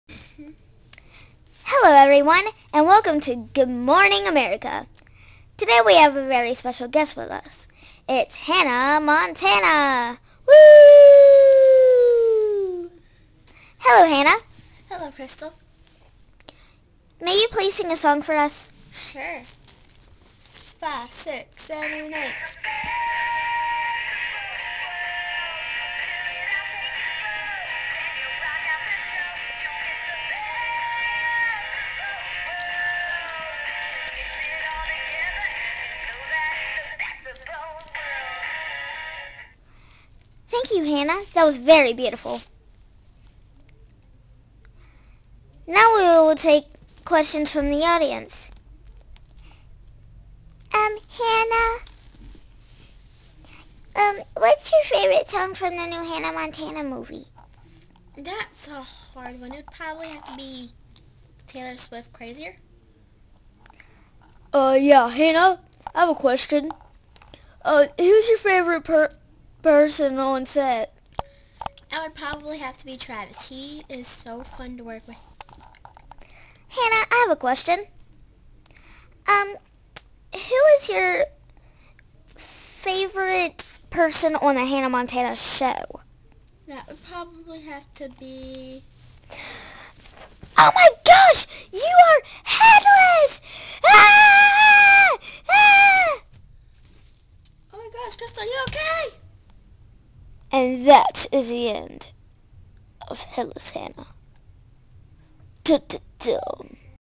HEADLESS Hannah
Watch the doll version hannah head fall off its body during a doll morning talk show!